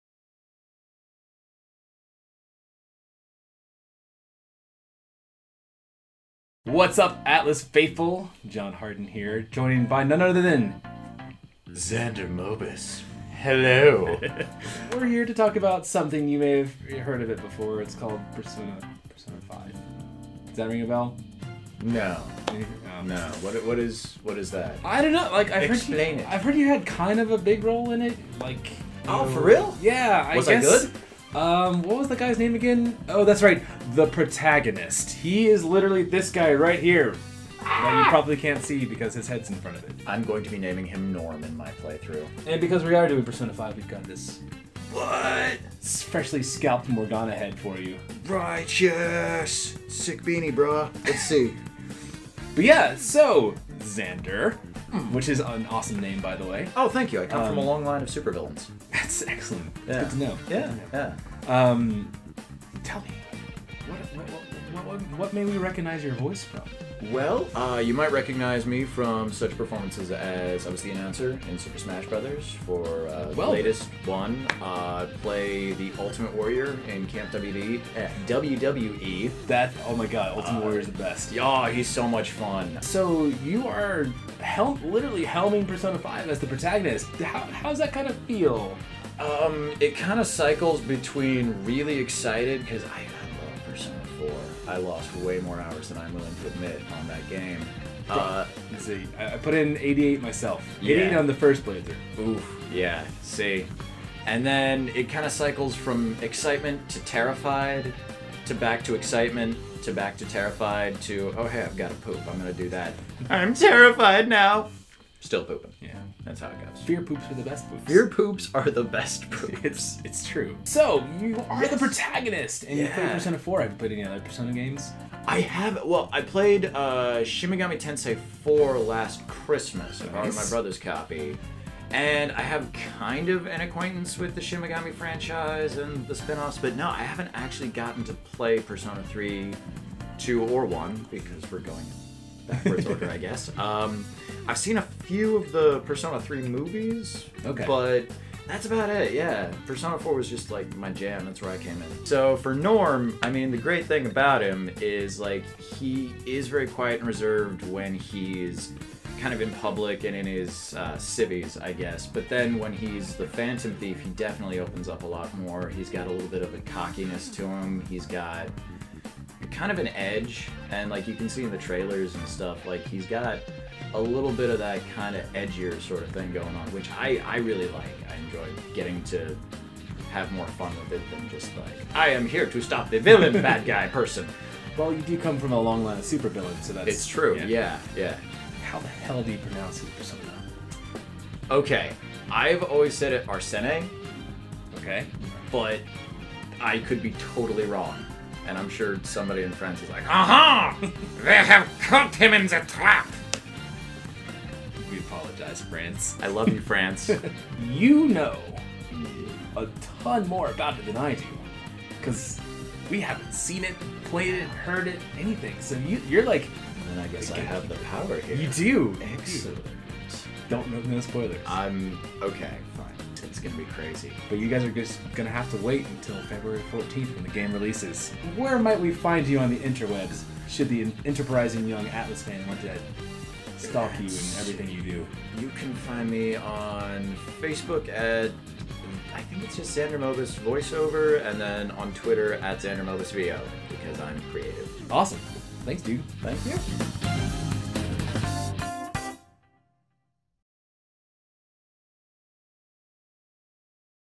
Persona 5 - Xander Mobus On Playing the Protagonist | PS4 PS3
Xander talks about some of his recent work, what it’s like portraying the protagonist, his experience with Persona, and so much more in this Q&A video.

Persona 5 is a game about the internal and external conflicts of a group of troubled youth who live dual lives. They have the typically ordinary day-to-day of a Tokyo high-schooler – attending class, after school activities and part-time jobs. But they also undertake fantastical adventures by using otherworldly powers to enter the hearts of people. Their power comes from the Persona, the Jungian concept of the “self;” the game’s heroes realize that society forces people to wear masks to protect their inner vulnerabilities, and by literally ripping off their protective mask and confronting their inner selves do the heroes awaken their inner power, and use it to strive to help those in need. Ultimately, the group of Phantom Thieves seeks to change their day-to-day world to match their perception and see through the masks modern-day society wears.

Persona 5 will release on Feb. 14 in the Americas and Europe, for PlayStation 4 and PlayStation 3. Pre-orders are now available at participating retailers for either version of the game. The game is rated M by the ESRB.